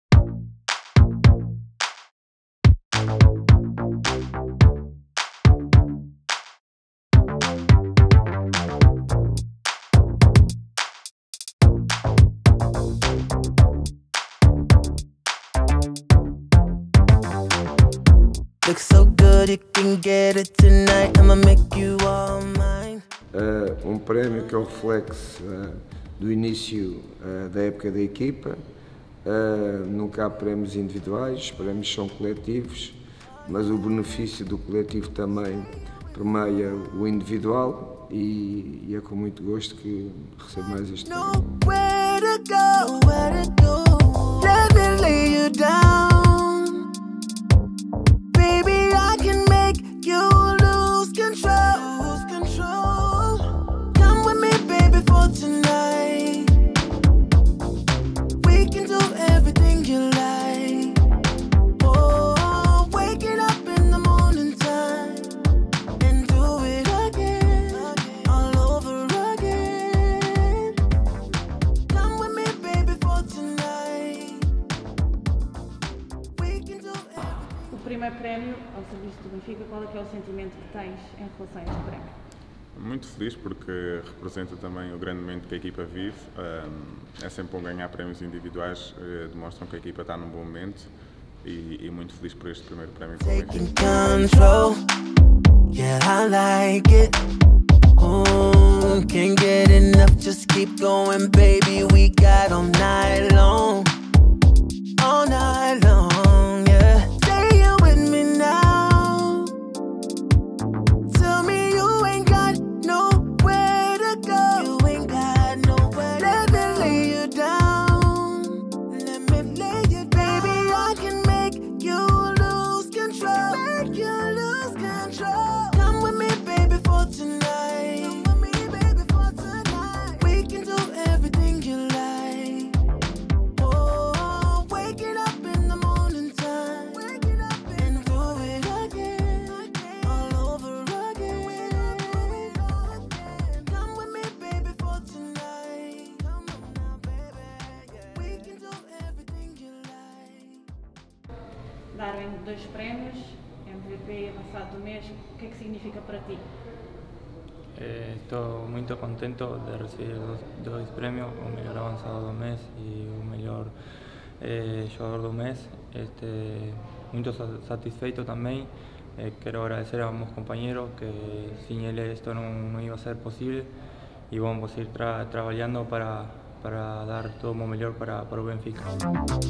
OS MELHORES DE SETEMBRO!
Jorge Jesus, João Mário e Darwin receberam, respetivamente, os prémios de Melhor Treinador, Melhor Médio, e Melhor Avançado e Melhor Jogador do mês de setembro.